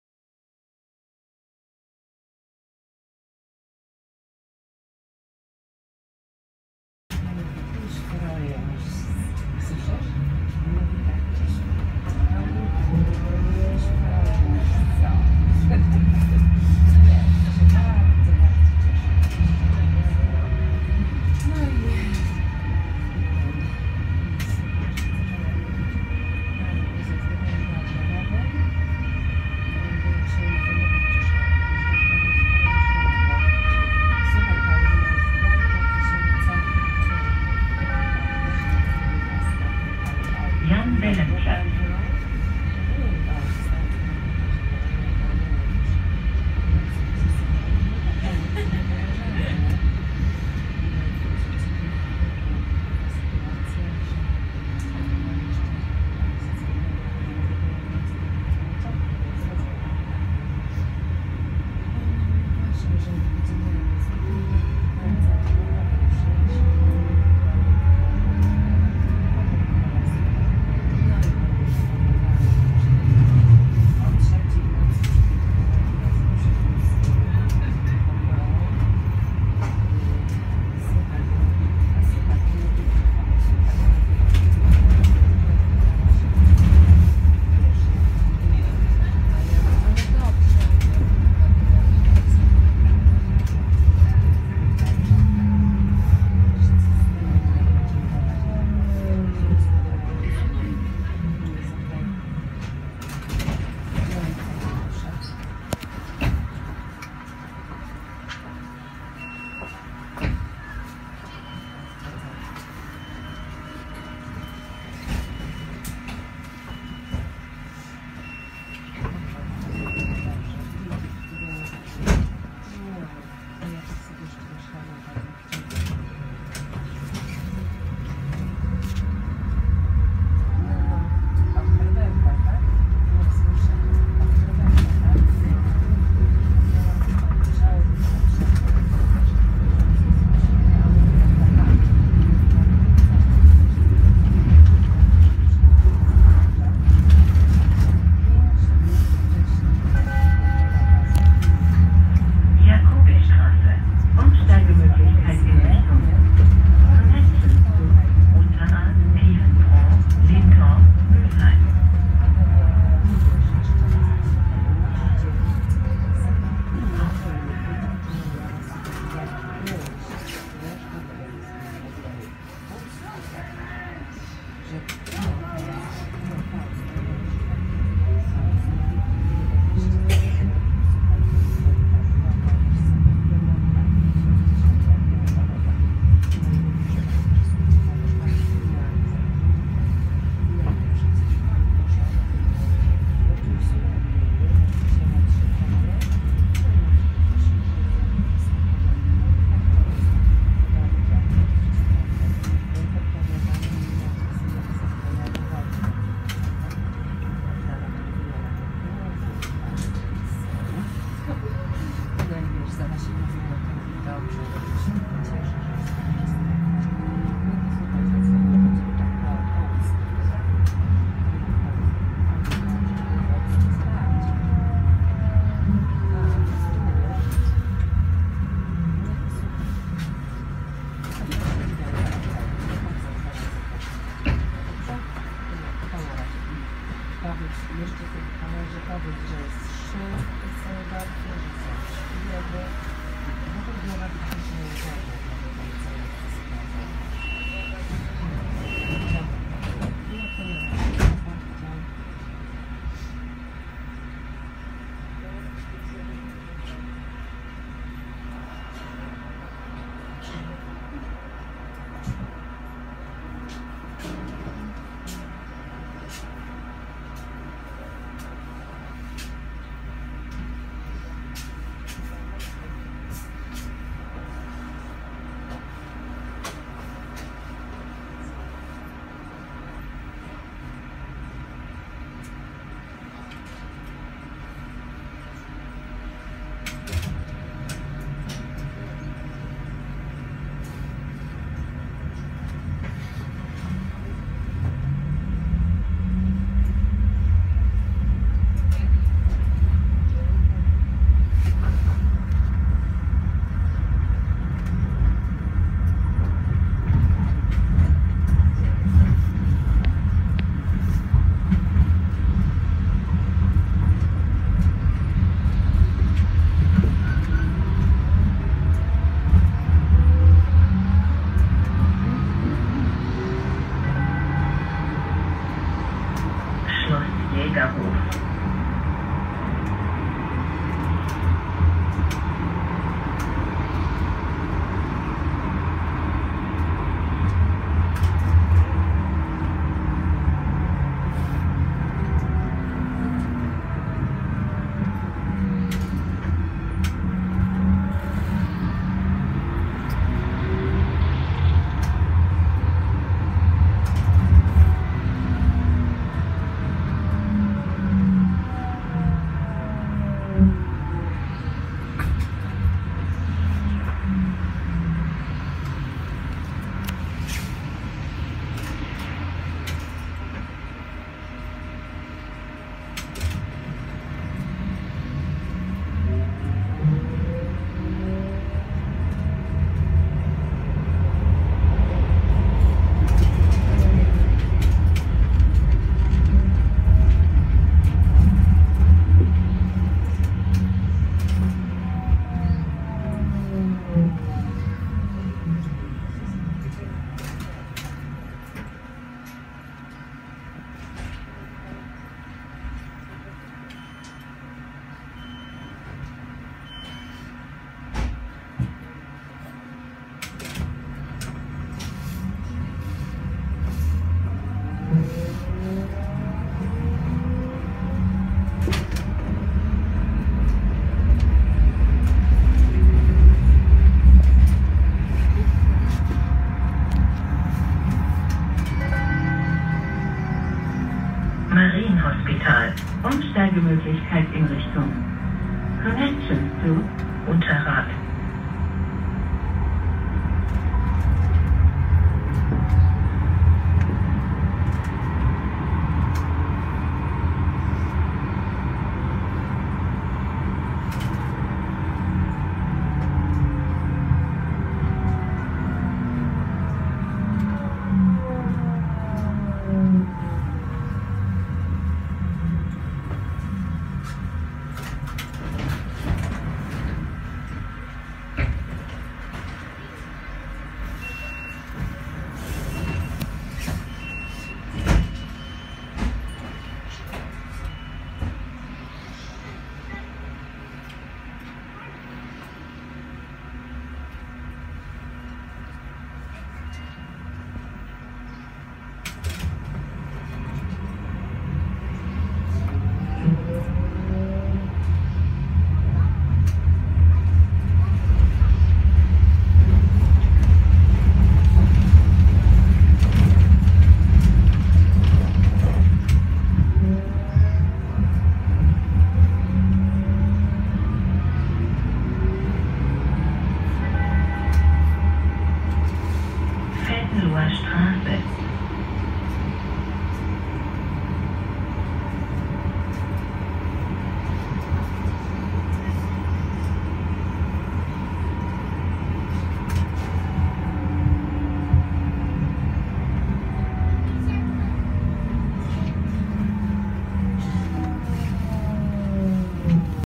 Mitfahrt auf der 715 Februar 2016
Kurz vor Eröffnung der Wehrhahnlinie  eine Mitfahrt gefilmt durch das Heckfenster eines NF6 Triebwagens. Start ist die Haltestelle Heinrich-Heine-Allee, Ende des Films die Haltestelle Venloer Straße.